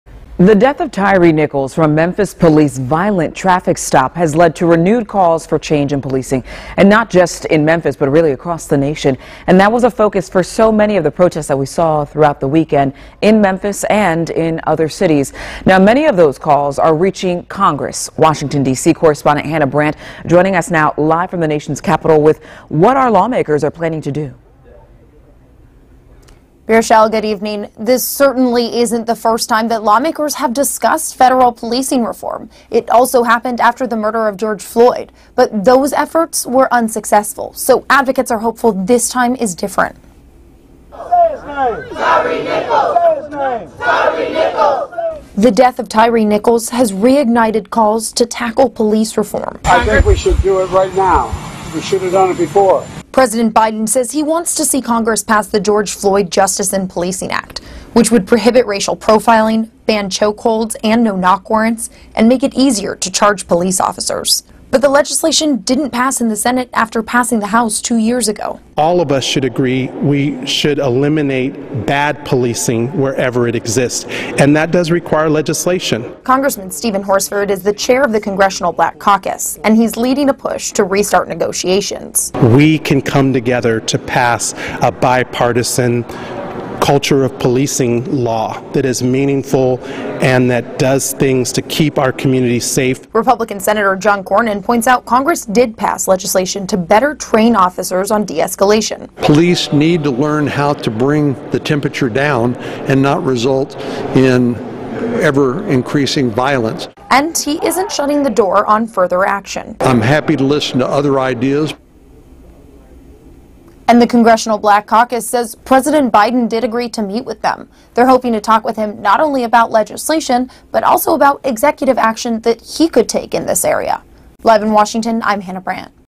Death of Tyre Nichols reignites calls for federal policing reform
Congress is discussing police reform in the wake of the death of Tyre Nichols during a violent police stop in Memphis, Tennessee.